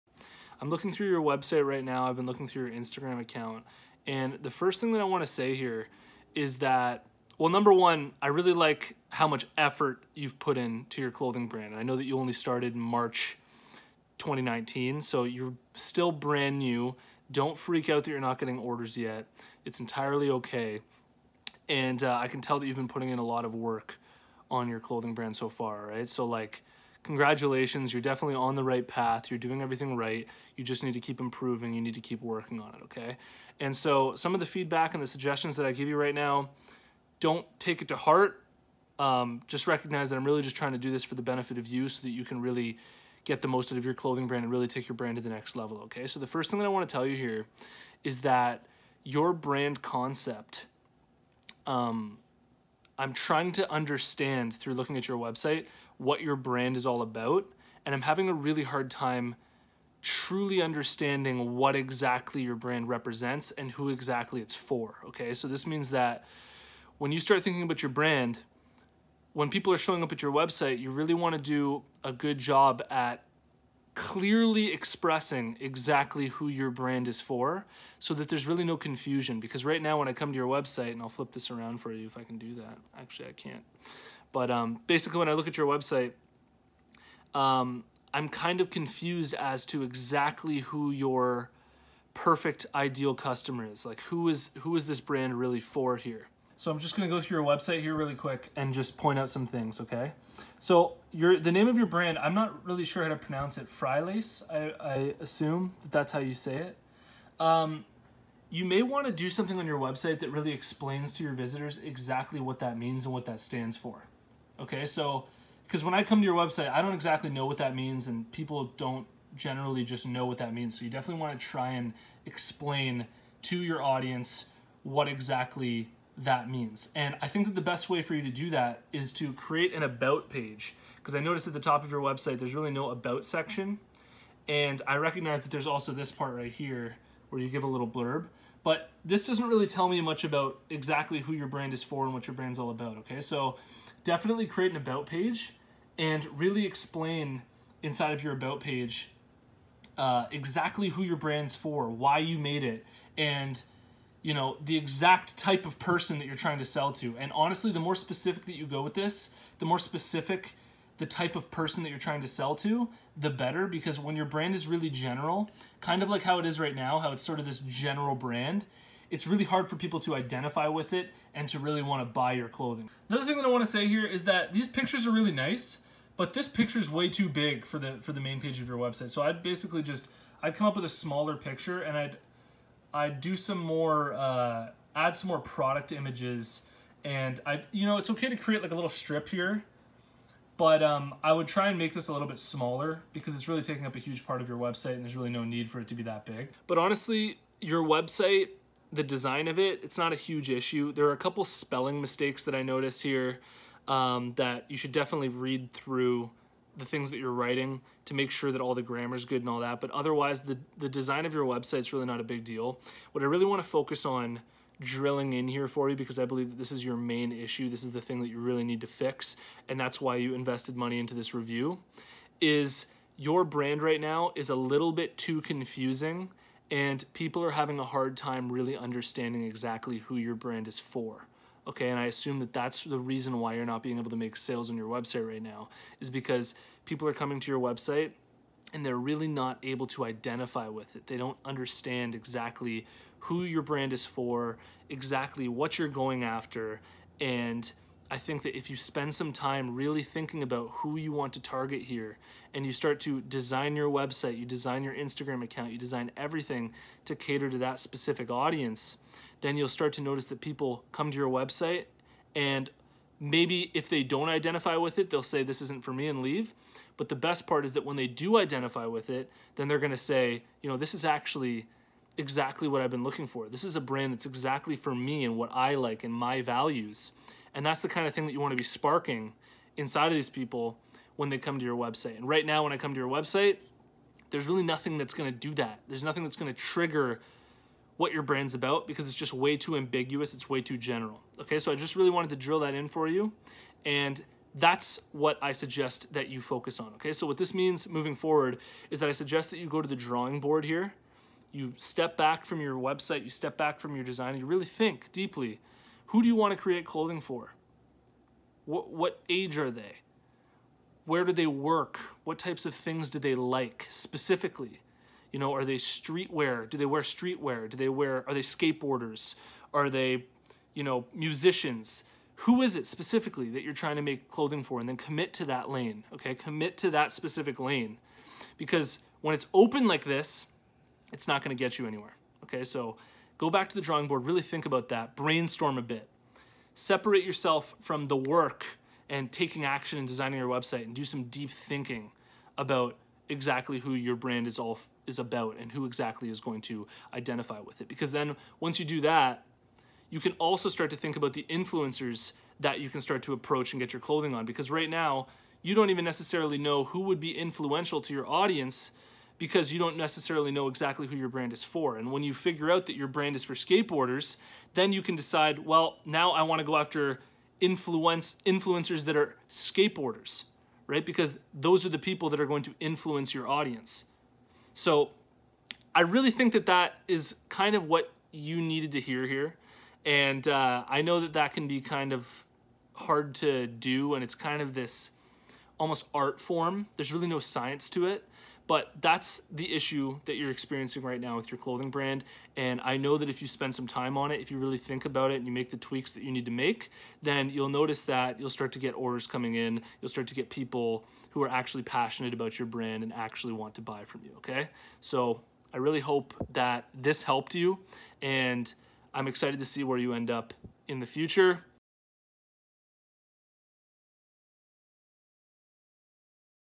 Clothing Brand Review: Frilace Clothing (November 8th, 2019)
This is a PAID clothing brand review for Frilace Clothing. The owner agreed to having this review shared to the YouTube channel. If you would like your clothing brand reviewed by Rob, you can sign up